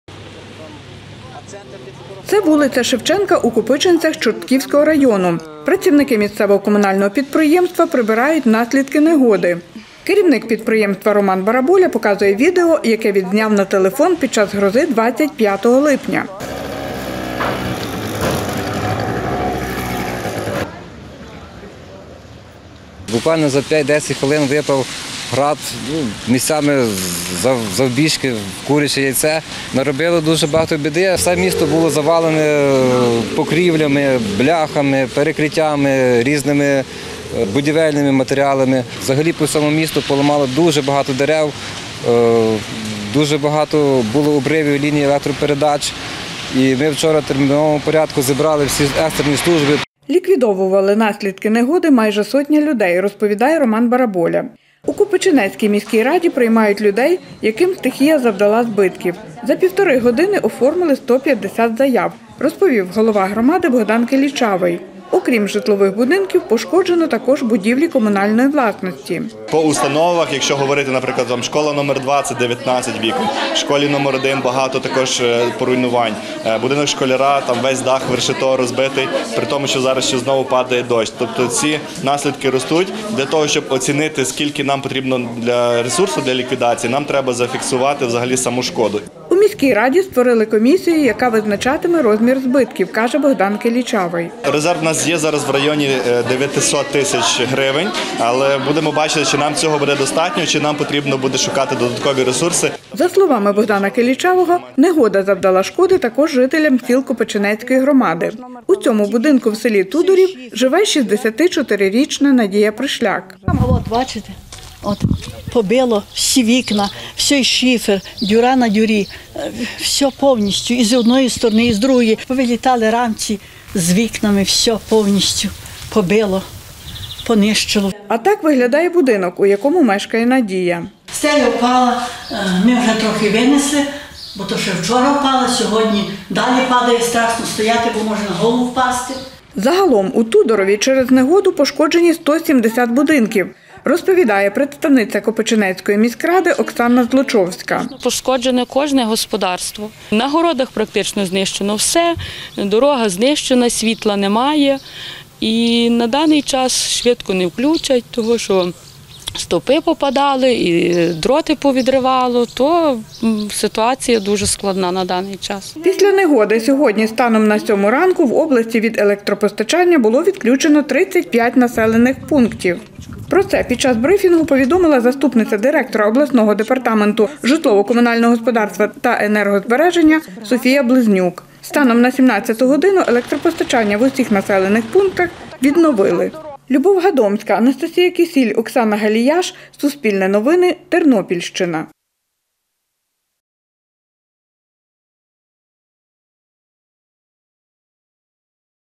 «Все побило, понищило»: наслідки негоди
Зірвані дахи, зруйновані будинки, повалені дерева та електроопори. Такі наслідки негоди у Копичинецькій громаді Чортківського району. Гроза пройшла 25 липня. У міській раді створили спеціальну комісію, яка фіксує збитки та підраховує шкоду, завдану приватним господарствам та комунальним установам міста. Про це розповів голова Копичинецької громади Богдан Келічавий.